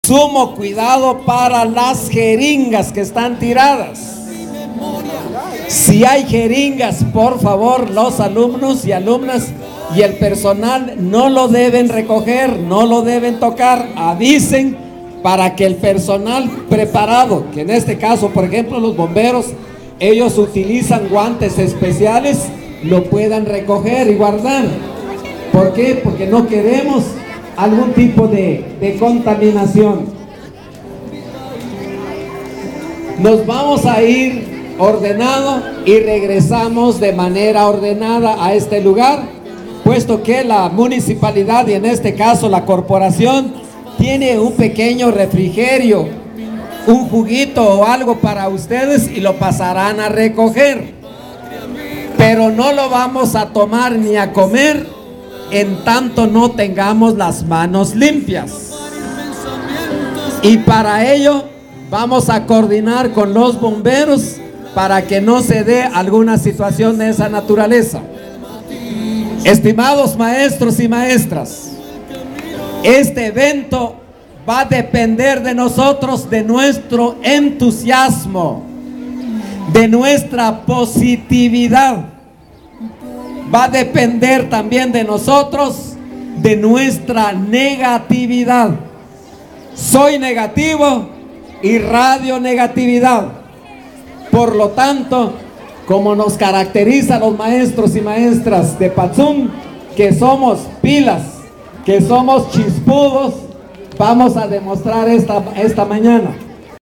Campaña recogida de basura con centros educativos
Patzún, Campaña recogida de basura con centros educativos organizada por la Municipalidad en coordinación con la Supervisión Educativa de Patzún